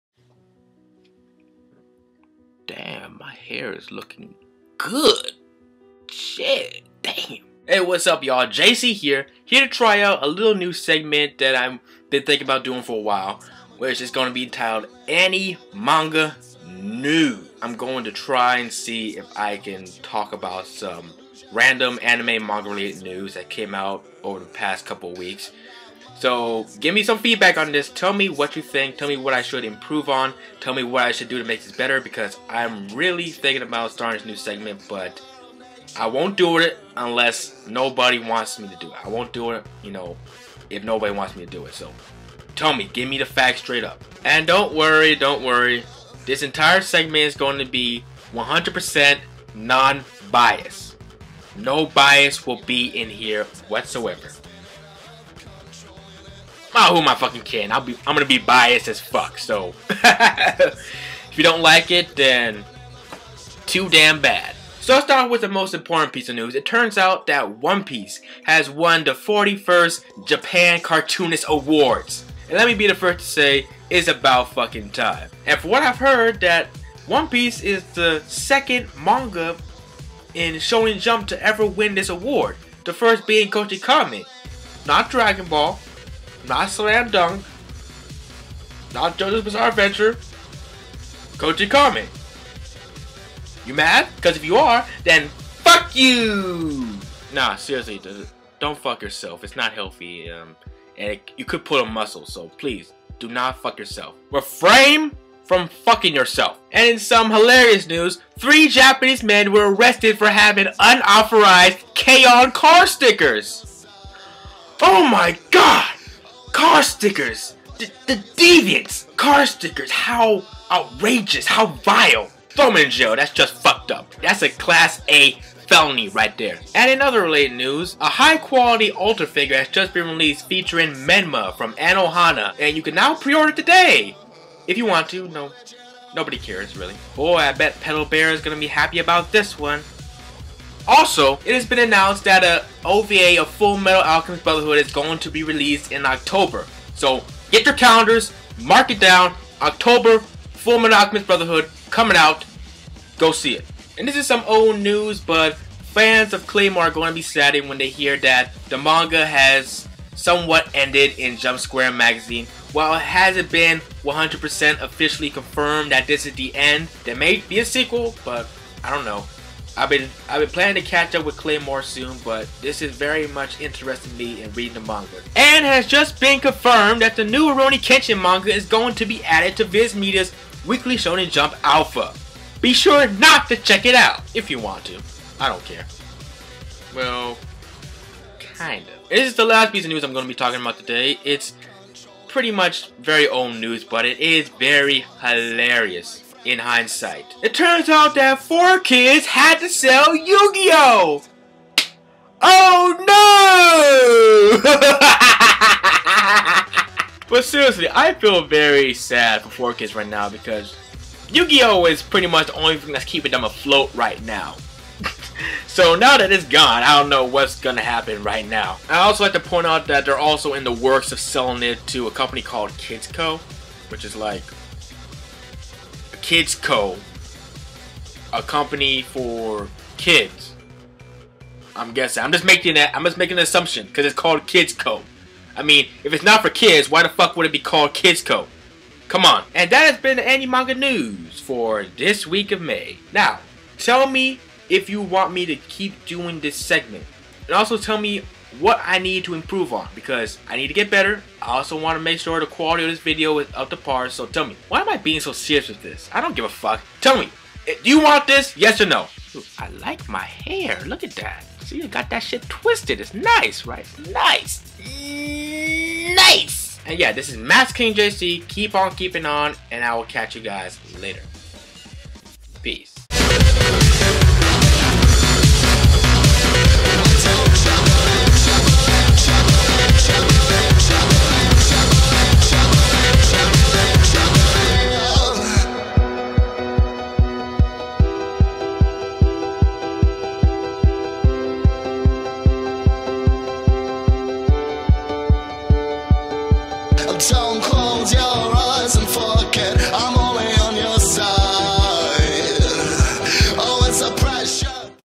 One Piece Wins Japan Cartoonist Award, FMA Brotherhood OVA, 4Kids & YU-GI-OH (AniManga News)
Links:
One Piece Wins 41st Japan Cartoonist Award: